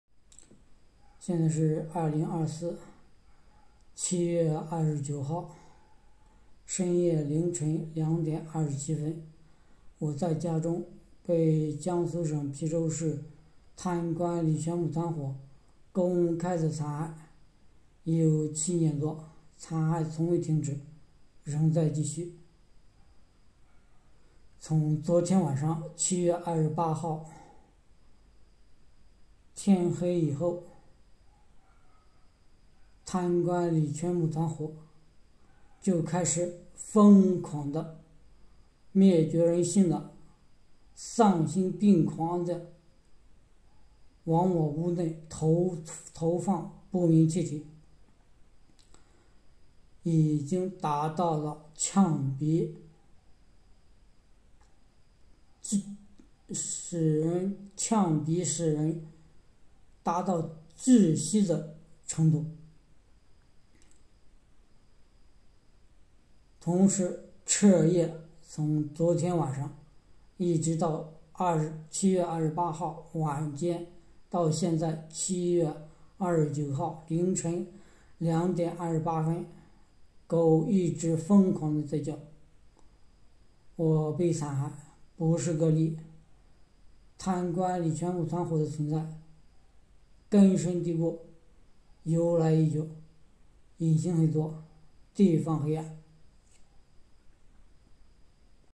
VID 20240729 022715 - 深夜凌晨2点屋内再次出现呛鼻的气味,严重到可以窒息,造成胸腔内强烈的灼伤灼痛感,贪官李全普团伙根深蒂固,我被残害不是个例,隐情很多,地方黑暗
VID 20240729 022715 -深夜凌晨2点屋内再次出现呛鼻的气味,严重到可以窒息,造成胸腔内强烈灼伤灼痛感
我在家中被江苏省邳州市贪官李全普团伙公开残害已七年多,残害从未停止,仍在继续
长年累月长达七年多,如复一日,每天24小时对我实施残害
我被残害不是个例,贪官李全普团伙根深蒂固,隐情很多,地方黑暗